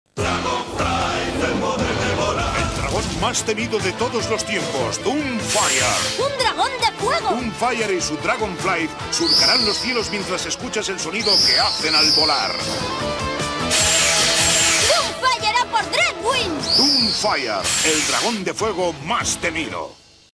Dragon Flyz Doomfyre (Anuncio de Juguetes Famosa)
Un Anuncio de Juguetes de La Serie Animada Dragon Flyz de Famosa